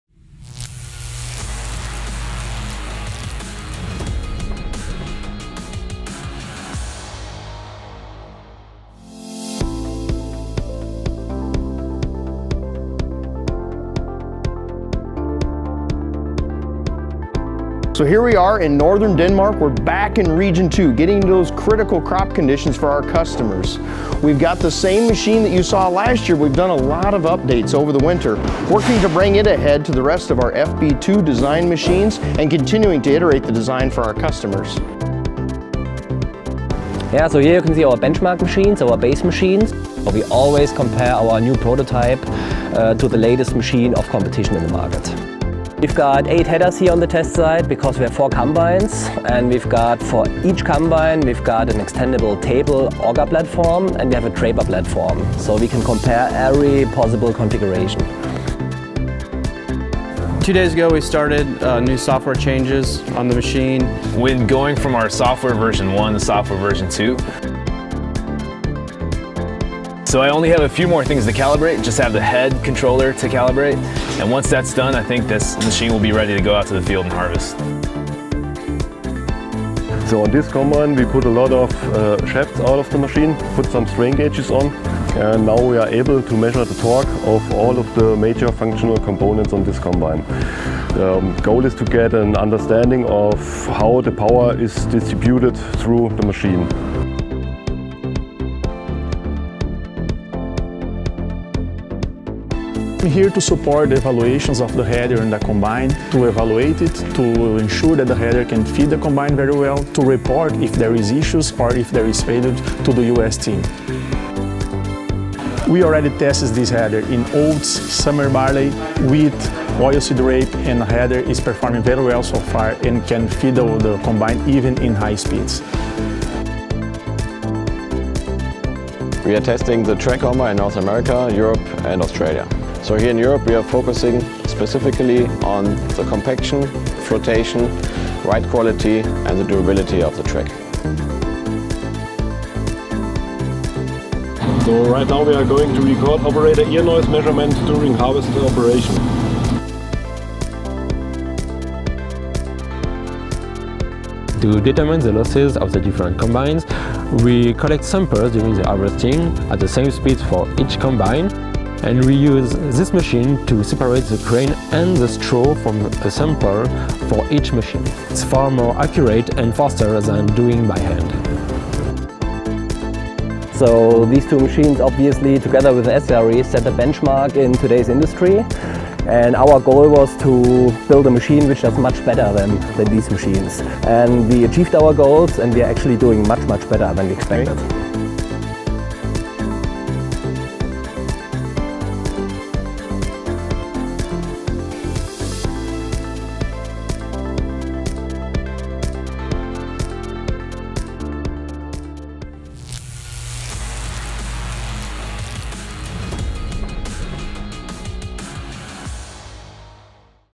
Xtreme testing - the new X9 Harvester in Denmark
"Xtreme testing - The new X9 in the field, higher throughput, lower fuel consumption.  John Deere Chief Engineer Jason Healy and our expert team head to Denmark.
Be prepared for an experience that challenges what you have ever thought about harvesting. Higher throughput. Lower fuel consumption. More comfort. Increased automation. And lots more!
Models shown are pre-production, test equipment.  Specifications and design of production models are subject to change.